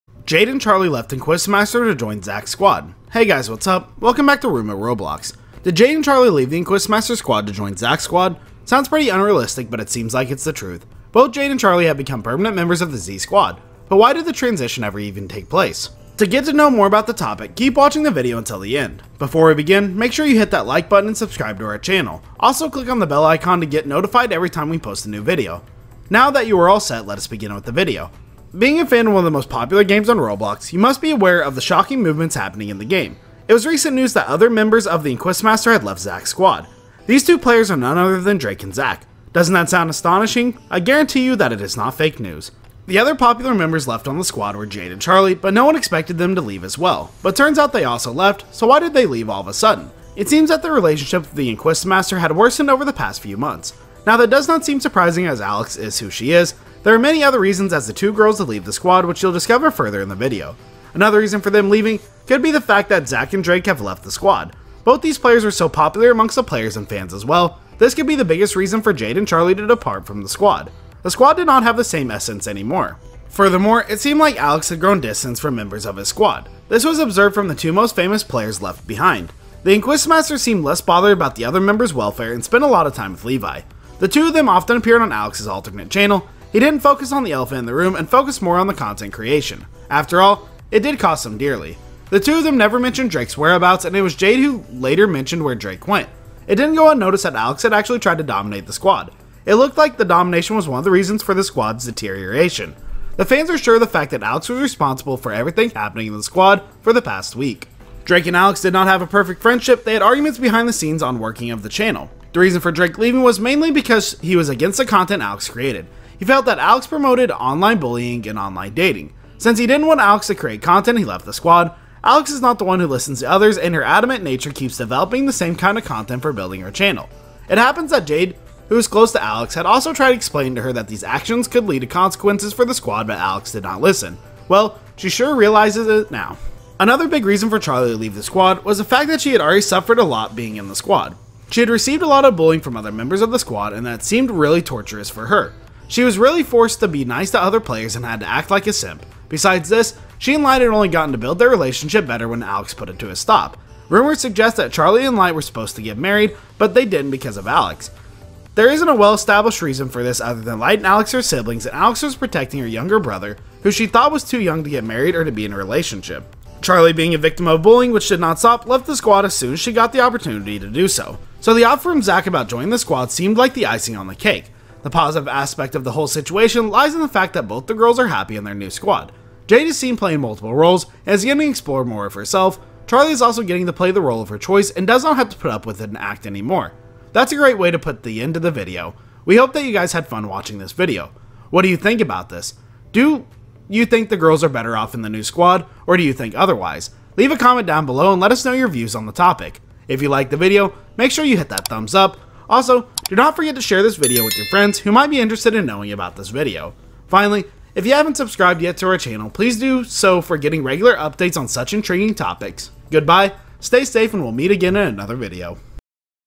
Jade and Charli Left The Inquisitormaster Squad To Join... THE ZACH PACK?!
More ZACHPACK Drama Don't Miss Out!

Don't forget to check more content BELOW!